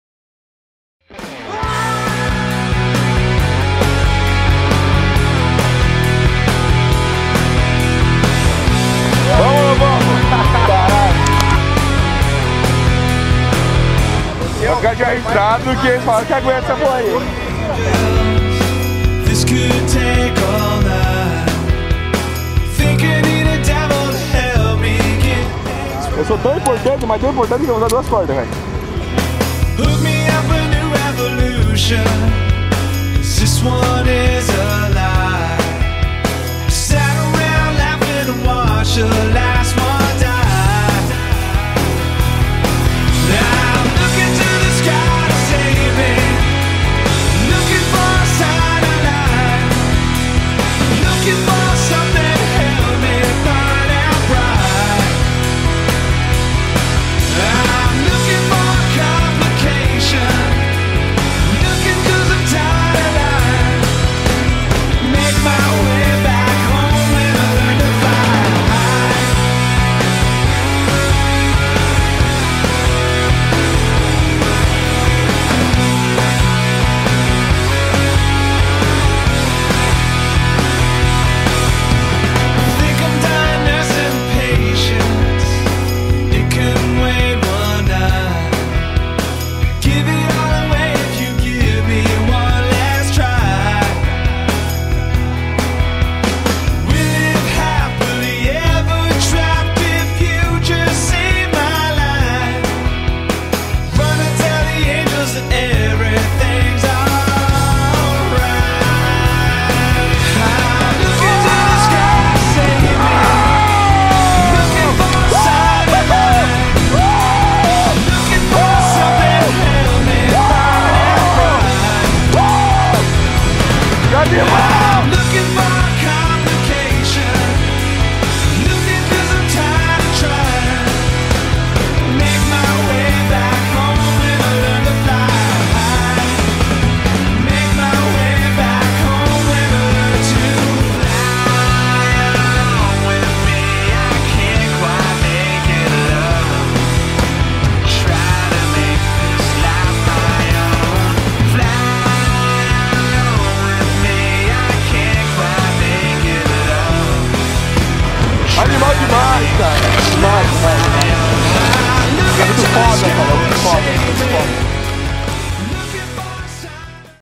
010. Diego Pereira (06.10.2013) Master Jump
Salto de Bungee Jumping a 42m de altura.